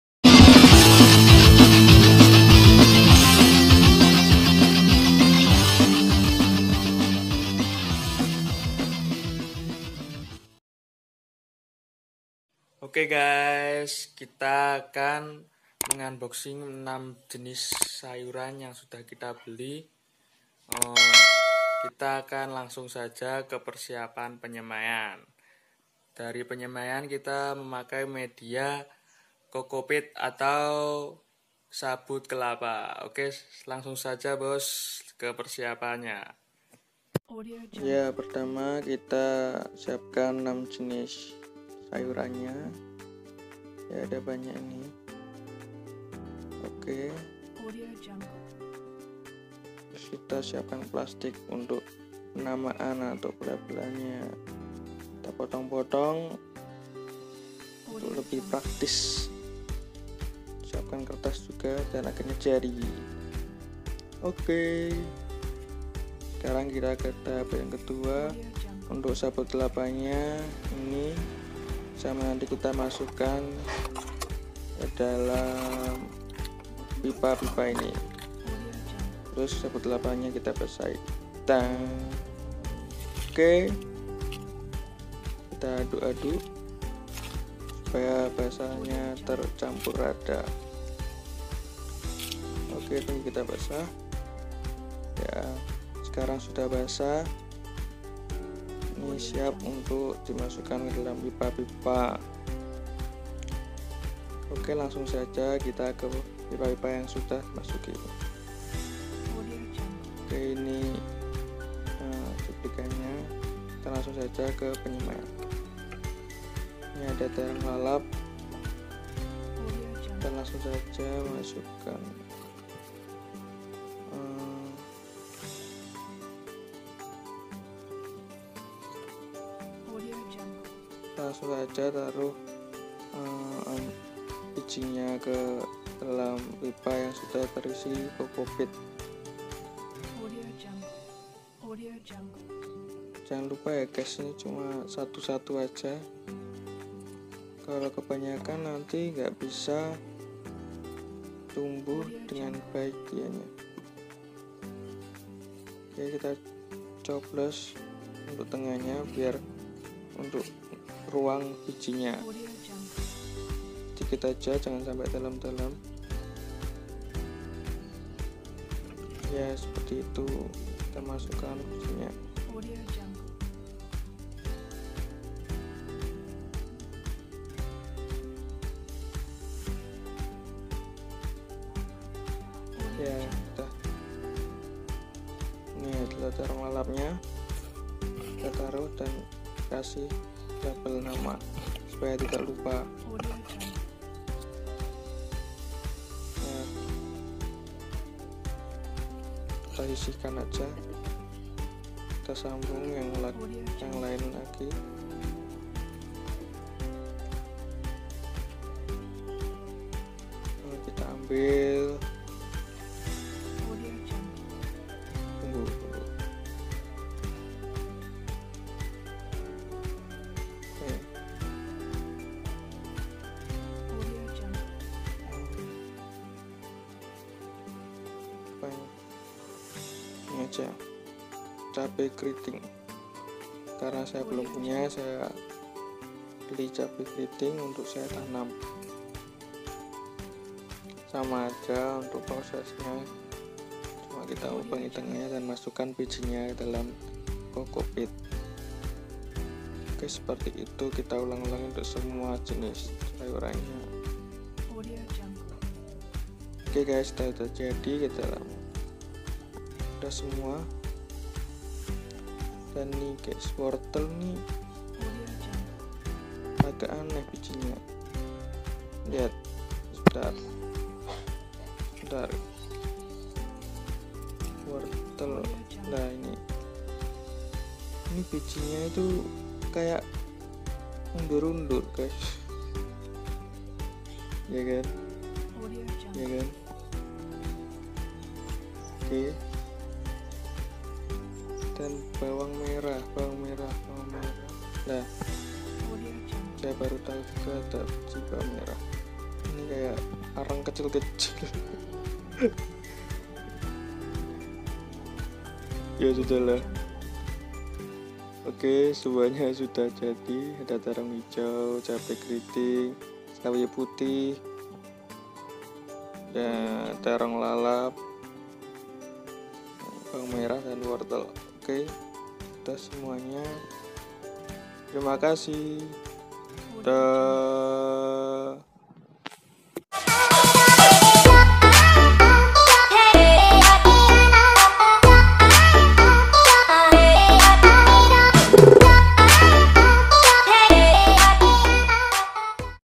#2 PENYEMAIAN DENGAN COCOPEAT? + UNBOXING 6 JENIS BIJI SAYUR
Haii gaes...
Video kali ini saya akan menyemai biji wortel, bawang merah, terong lalap, terong hijau, cabe keriting, dan sawi putih yg sudah saya beli dengan media sabut kelapa/cocopeat.

Semoga bermanfaat bagi kita semua